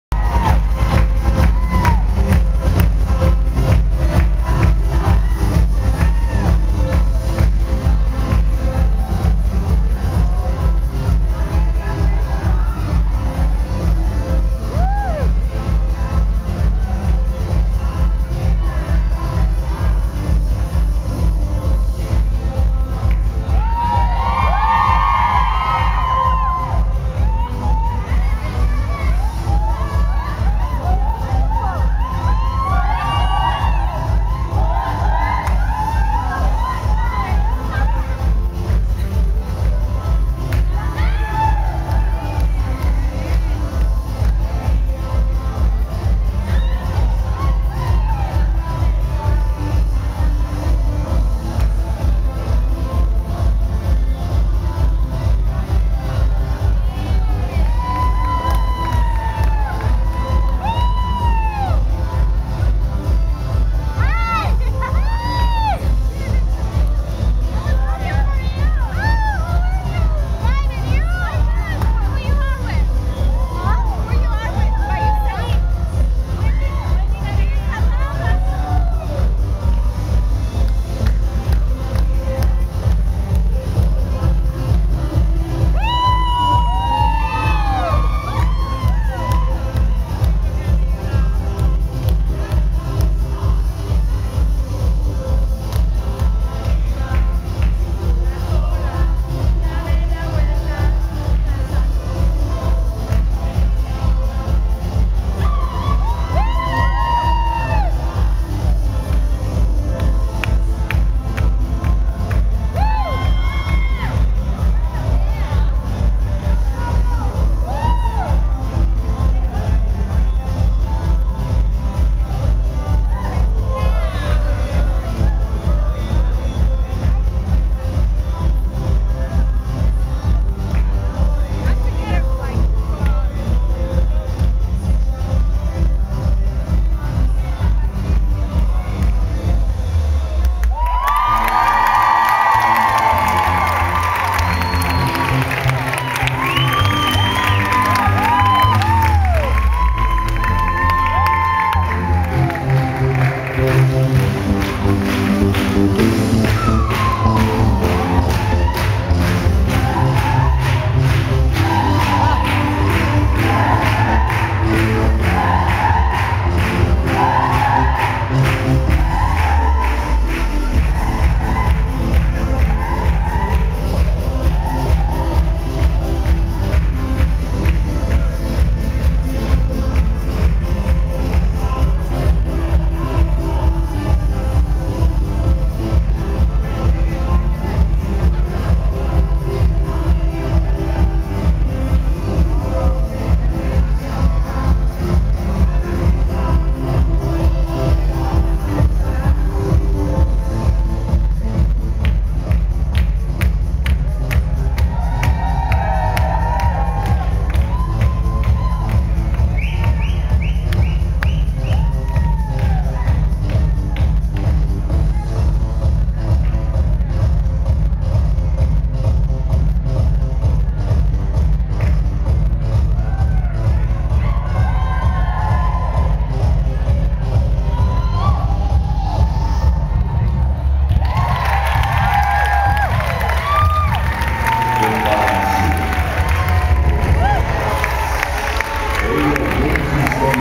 Red Carpet Event
Me presenting at the Red Carpet event in Orlando, FL right before the  2011 Zumba Fitness® Convention.